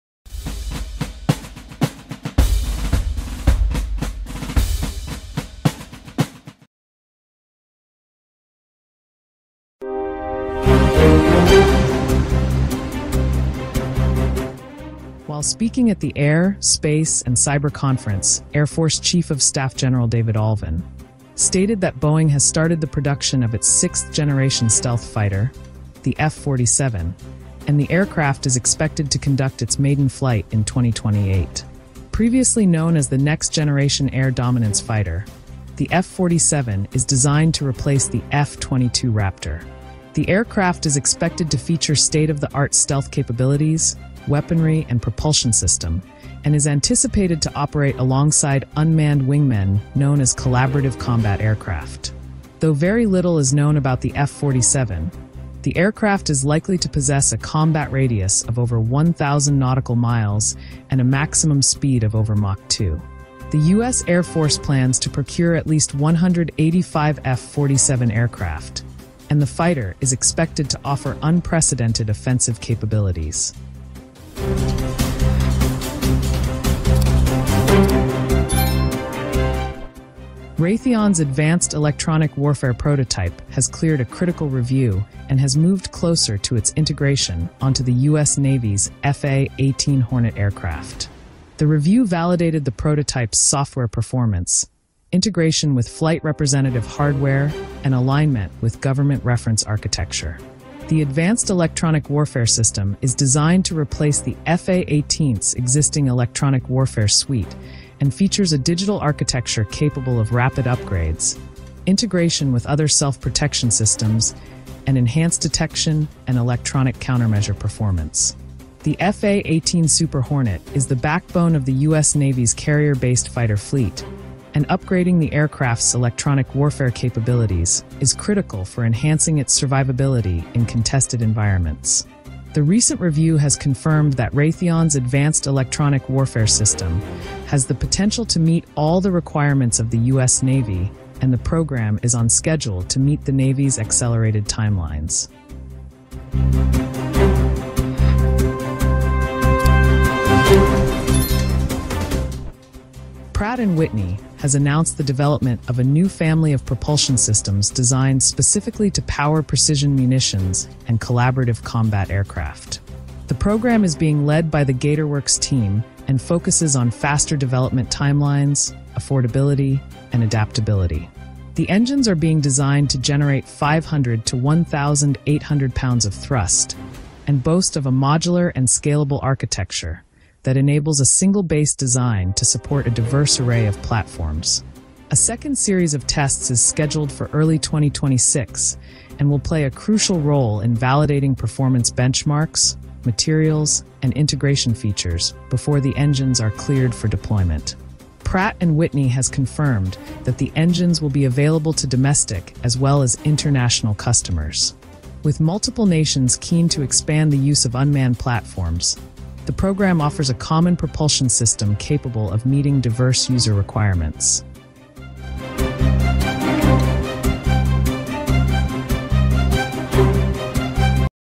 The United States Defense News - Boeing's F-47 enters prodction, Raytheon's FA-18 EW clears trials.
The United States Defense News for the 24th of Sept 2025.

1) Boeing's F-47 6th gen fighter enters production.

2) Raytheon’s EW prototype for FA18 aircraft clears trials.

3) Pratt & Whitney develop new engines for unmanned aircraft.

USA # United States Of America # US Army US Air Force # US Marines # US Navy Boeing F47 6th Gen Fighter Raytheon FA-18